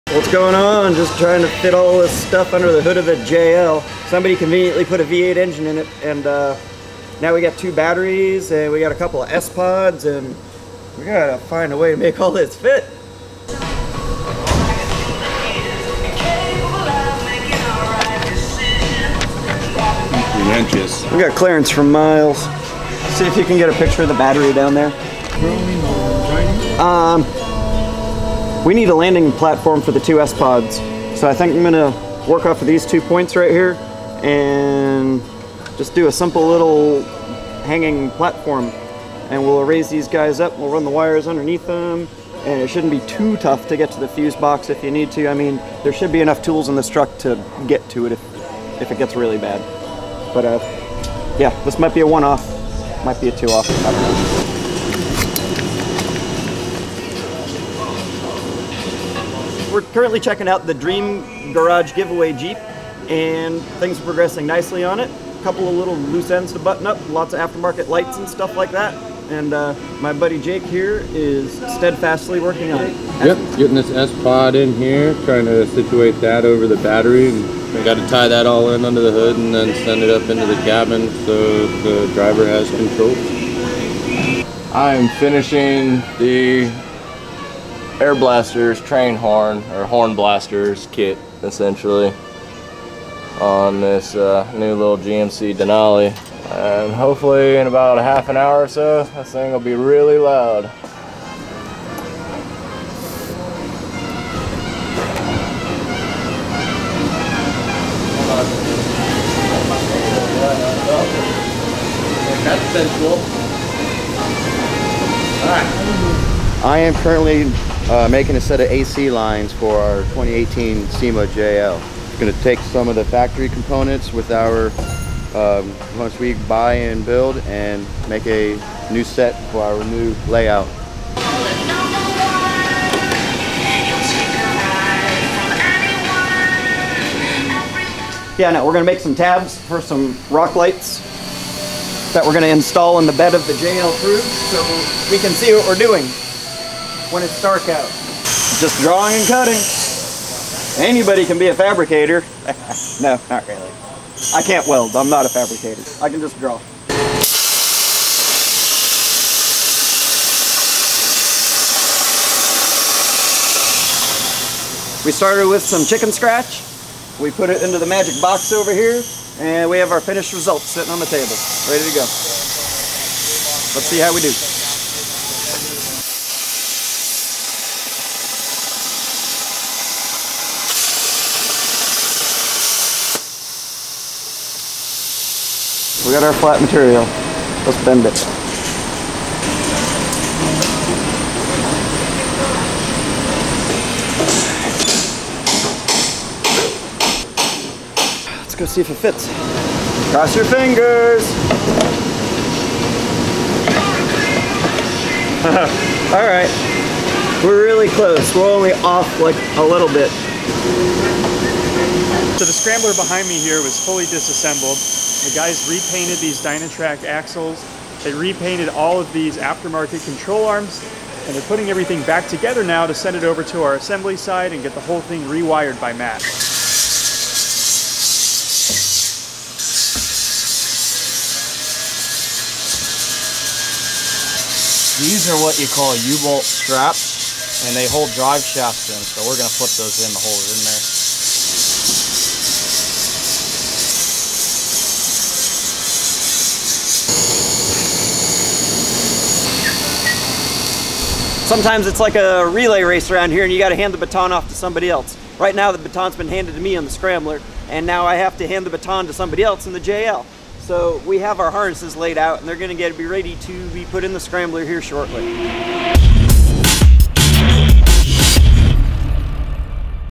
Bruiser Vlog #2: JL Crew, Bruiser JL, Scrambler and SEMA progress.
The Team has been working hard and in a very well coordinated manner at a fast pace; here is a substantial update on the JLCrew, the BruiserJL & Dream Giveaway Sweepstakes Jeeps...

Also, take a look at how the Scrambler has been upgraded, and how Matt develops a custom mount for the Double sPOD setup for the JLCrew.